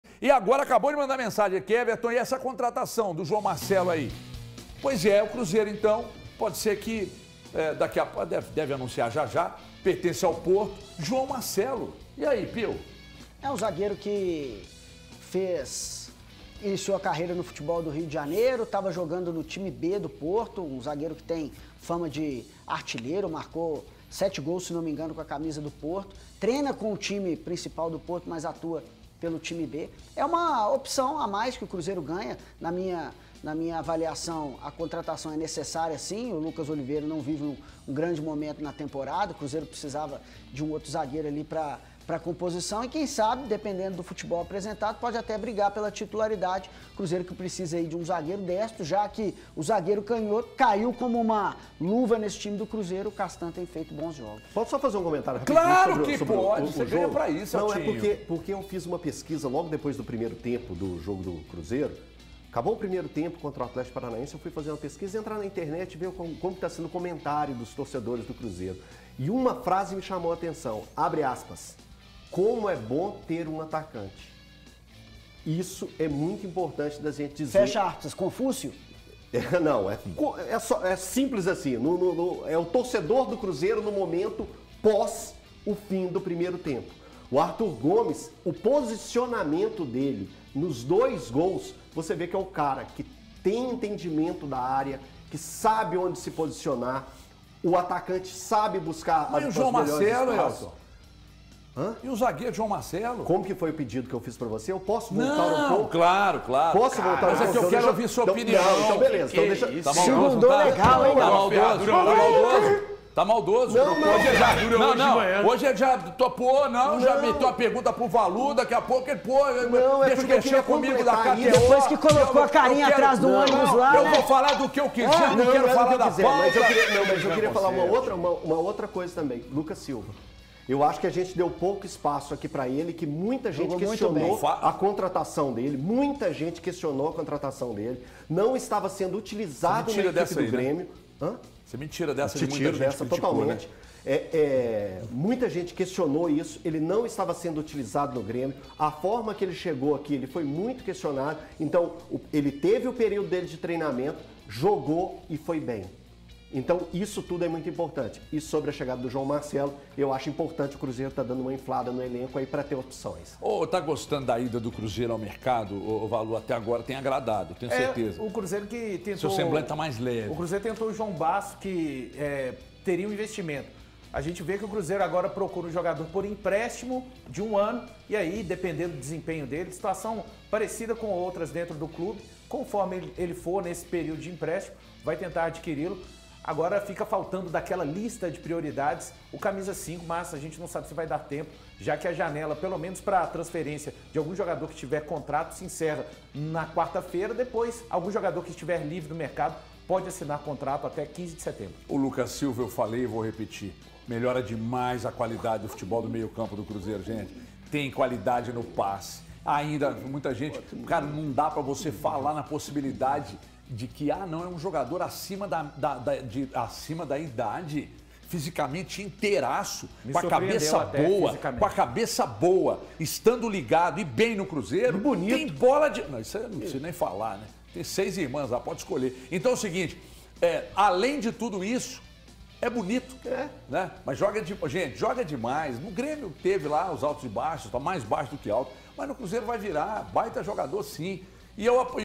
João Marcelo deve reforçar o Cruzeiro. Zagueiro chega por empréstimo!
O Cruzeiro se aproxima da contratação do zagueiro João Marcelo, de 23 anos. O atleta estava no Porto B e teve ótimos números na segunda divisão de Portugal. Chega pra ser titular?